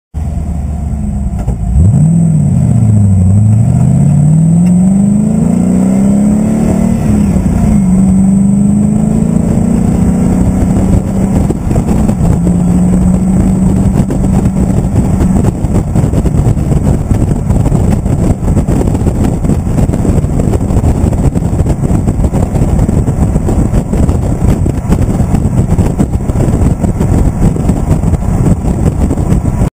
Cobra Santa
Santa digs my sled as I drive through Londonderry Town Square.